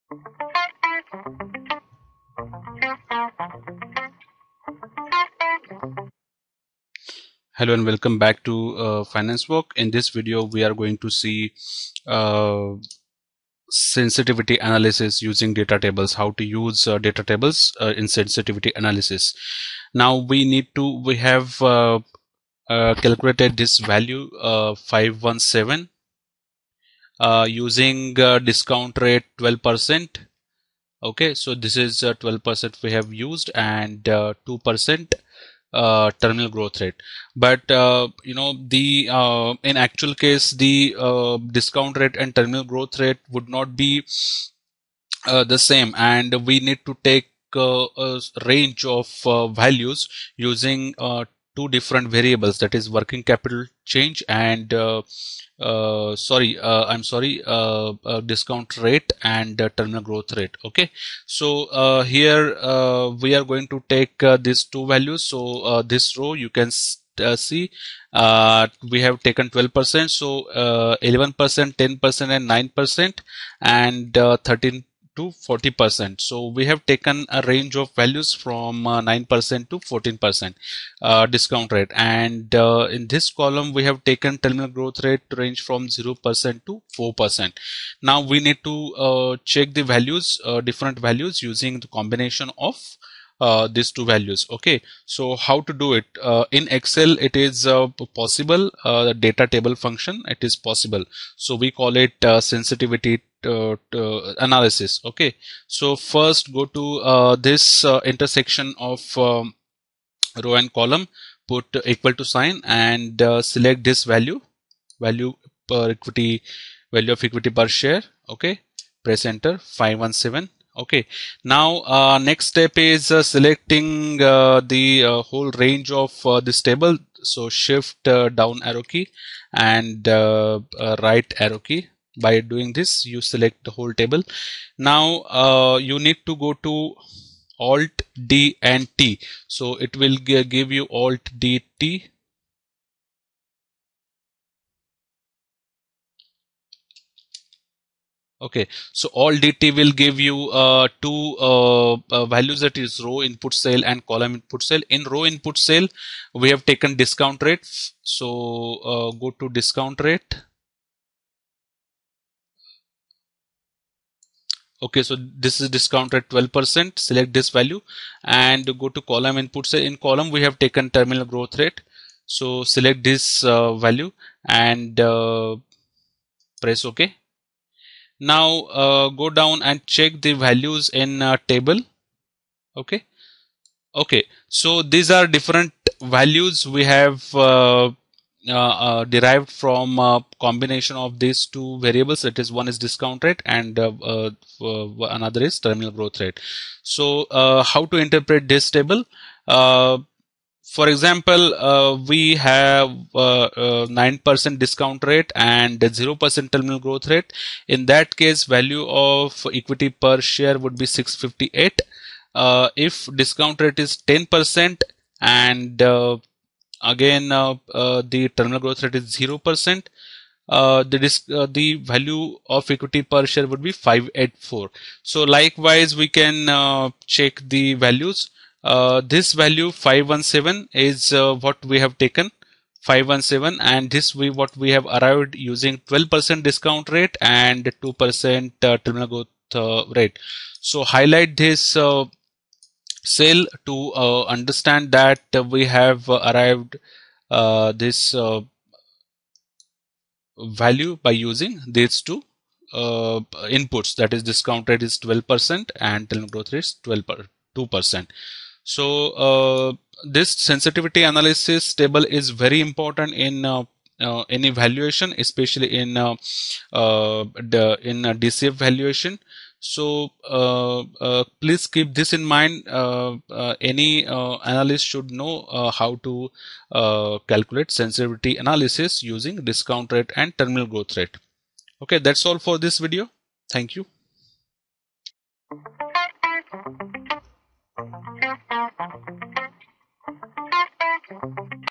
Step-By-Step Video on Sensitivity Analysis in Excel used in Financial Modeling | FinanceWalk
Chapters/Timestamps:

00:00 Intro to Sensitivity Analysis 📊
00:54 Discount Rate & Growth Rate Variables 📈
01:37 Data Table Function for Analysis 🧮
02:34 Input Cell Selection Process 🖱️
03:27 Interpreting Sensitivity Table Results 🔍
04:15 Importance in Valuation Models 💼